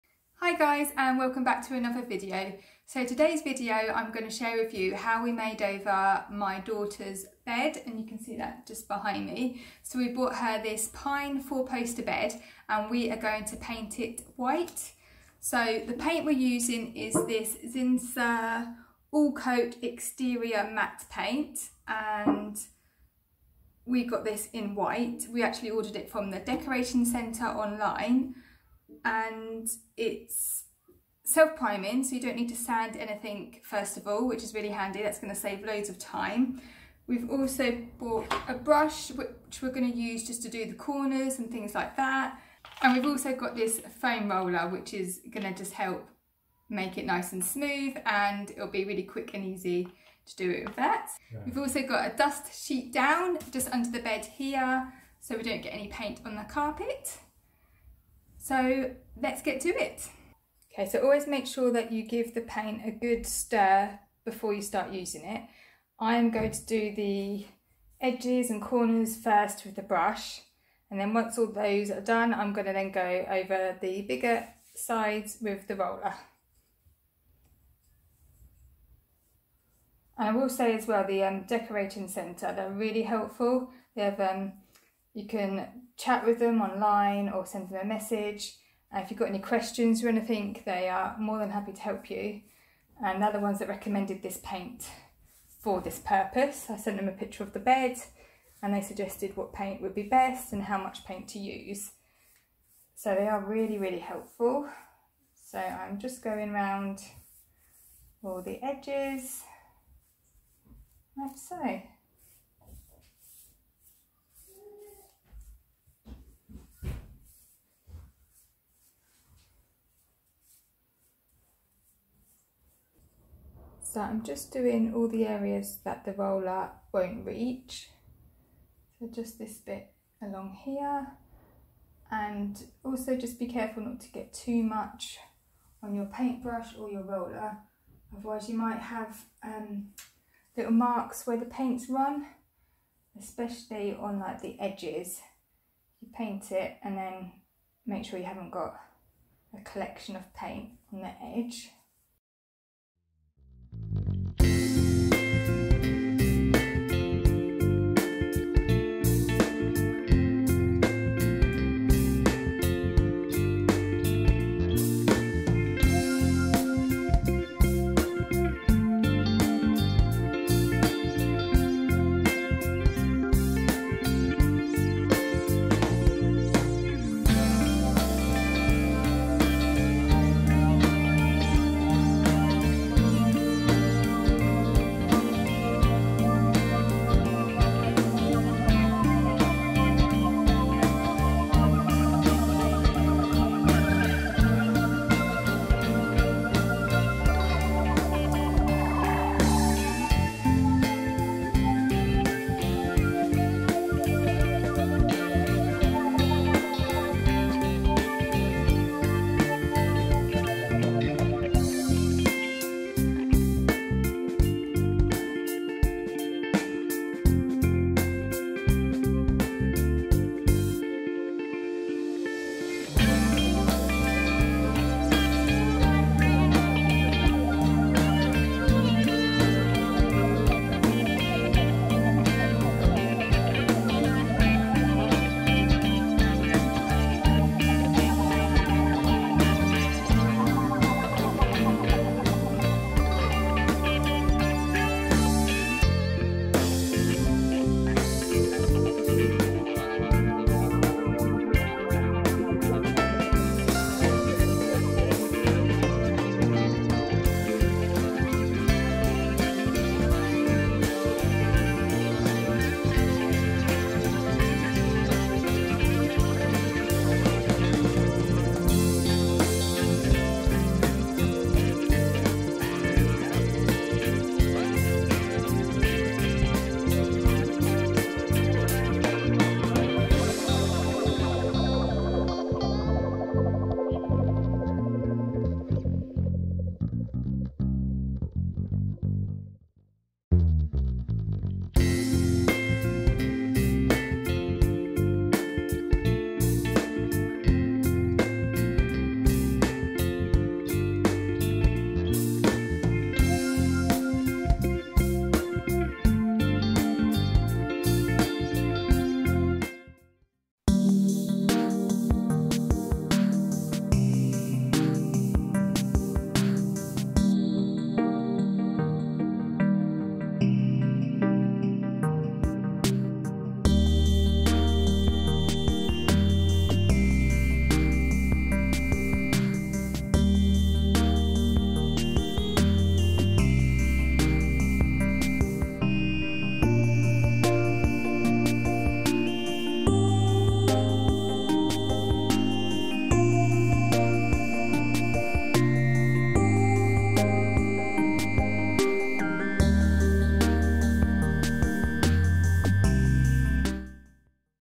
DIY BED MAKEOVER | How to paint furniture
Hi guys,

We decided to paint my daughter's four poster pine bed and I am so happy with the results! It's the first time I've tried to paint a large item of furniture, but the Decorating Centre were really helpful and so I thought I would give it a go. I will do a full room tour when her room is complete :)